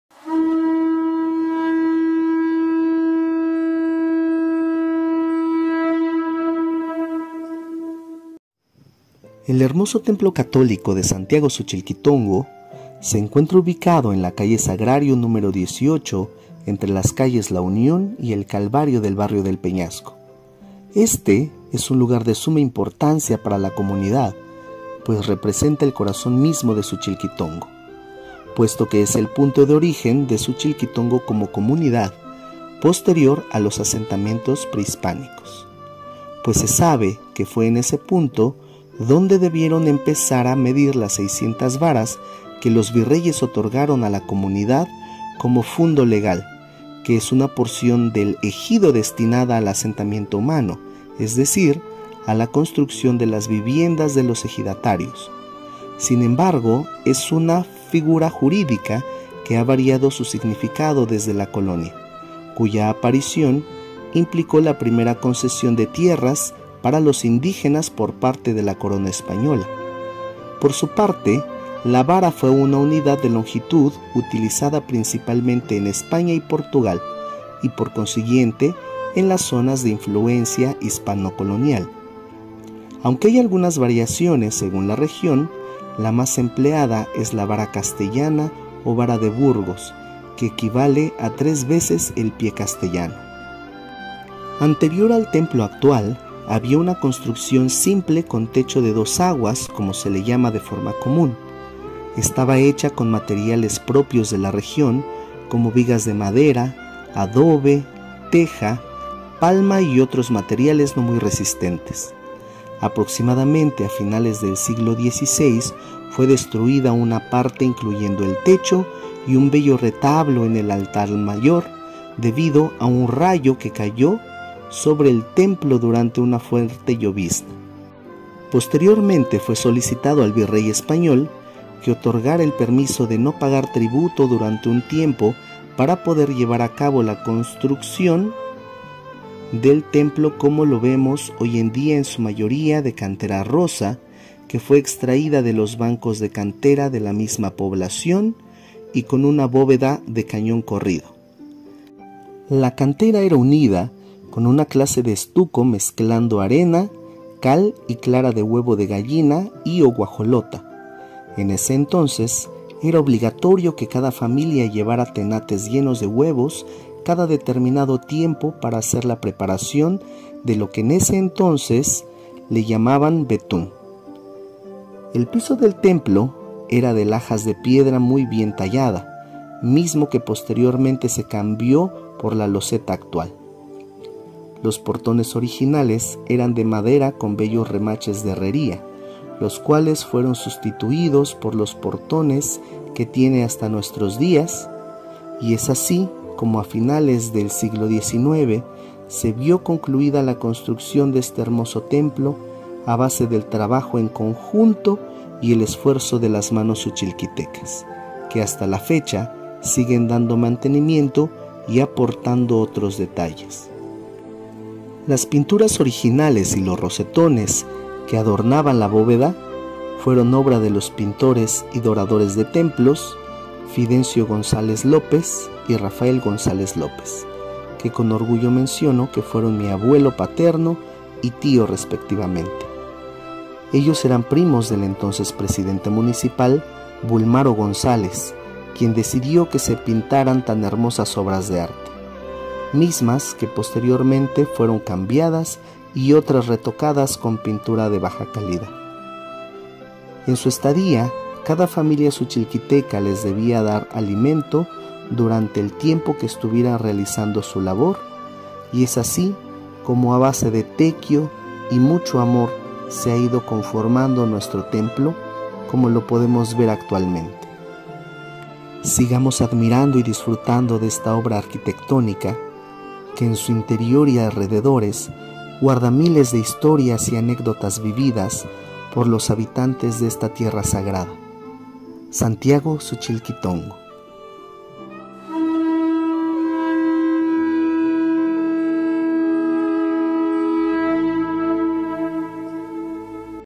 El hermoso TEMPLO CATÓLICO DE SANTIAGO SUCHILQUITONGO, ETLA, OAXACA.
El hermoso TEMPLO CATÓLICO de SANTIAGO SUCHILQUITONGO se encuentra ubicado en la calle Sagrario #18, entre las calles La Union y el Calvario del Barrio del Peñasco, éste es un lugar de suma importancia para la comunidad pues representa el corazón mismo de Suchilquitongo, puesto que es el punto de origen de Suchilquitongo como comunidad posterior a los asentamientos prehispánicos. Se sabe que fue en ese punto dónde debieron empezar a medir las 600 varas que el virrey otorgó a la comunidad como FUNDO LEGAL que es una porción del ejido destinada al asentamiento humano, es decir a la construcción de las viviendas de los ejidatarios, sin embargo, es una figura jurídica que ha variado su significado desde la colonia, cuya aparición implicó la primera concesión de tierras para los indígenas por parte de la Corona española, por su parte la Vara fue una unidad de longitud utilizada principalmente en España y Portugal y por consiguiente en las zonas de influencia hispano colonial. Aunque hay algunas variaciones según la región, la más empleada era la vara castellana o vara de Burgos que equivale a 0,835905 m, tres veces el pie castellano de 0,278635 m.

Anterior al templo actual había una construcción simple con techo de dos aguas, como se le llama de forma común, estaba hecha con materiales propios de la región como vigas de madera, adobe, teja, palma y otros materiales no muy resistentes, aproximadamente a finales del siglo XVI fue destruida una parte incluyendo el techo y un bello retablo en el altar mayor debido a un rayo que cayó sobre el templo durante una fuerte llovizna, posteriormente fue solicitado al virrey español que otorgara el permiso de no pagar tributo durante un tiempo para poder llevar a cabo la construcción del templo como lo vemos hoy en día en su mayoría de Cantera Rosa que fue extraida de los bancos de cantera de la misma población y con una Bóveda de Cañón corrido, la cantera era unida con una clase de estuco mezclando arena, cal y clara de huevo de gallina y/o guajolota, en ese entonces era obligatorio que cada familia llevara tenates llenos de huevos cada determinado tiempo para hacer la preparación de lo que en ese entonces le llamaban betún, el piso del templo era de lajas de piedra muy bien tallada, mismo que posteriormente se cambió por la loseta actual, los portones originales eran de madera con bellos remaches de herrería, los cuales fueron sustituidos por los portones que tiene hasta nuestros días y es así como durante el siglo XVIII se vio concluida la construcción de este hermoso templo a base del trabajo en conjunto y el esfuerzo de las manos suchilquitecas, que hasta la fecha siguen dando mantenimiento y aportando otros detalles. Las pinturas originales y los rosetones que adornaban la bóveda fueron obra de los pintores y doradores de templos Fidencio González López y Rafael González López, que con orgullo menciono que fueron mi abuelo paterno y tío respectivamente, ellos eran primos del entonces Presidente Municipal Bulmaro González quien decidió que se pintaran tan hermosas obras de arte, mismas que posteriormente fueron cambiadas y otras retocadas con pintura de baja calidad. En su estadía cada familia suchilquiteca les debía dar alimento durante el tiempo que estuvieron realizando su labor, y es así como a base de tequio y mucho amor se ha ido conformando nuestro templo como lo podemos ver actualmente.

Sigamos admirando y disfrutando de esta obra arquitectónica que en su interior y alrededores guarda miles de historias y anécdotas vividas por los habitantes de esta tierra sagrada, Santiago Suchilquitongo.

The beautiful CATHOLIC CHURCH of SANTIAGO SUCHILQUITONGO is located on Sagrario street #18, between La Union and Calvario streets in Peñasco neighborhood. This is a place of great importance for the community as it represents the very heart of Suchilquitongo, since It is the point of origin of Suchilquitongo as a community after the pre-Hispanic settlements. It is known that it was at this point that they should have begun to measure the 600 yards that the viceroy granted to the community as a LEGAL FUNDO, which is a portion of the ejido intended for human settlement, that is, for the construction of the ejidatarios' homes, however , is a legal figure that has changed its meaning since the colony, whose appearance implied the first concession of land for the indigenous people by the Spanish Crown, for its part the Vara was a unit of length used mainly in Spain and Portugal and by consequently in the areas of Hispanic colonial influence. Although there are some variations depending on the region, the most used was the Castilian vara or Burgos vara, which is equivalent to 0.835905 m, three times the Castilian foot of 0.278635 m.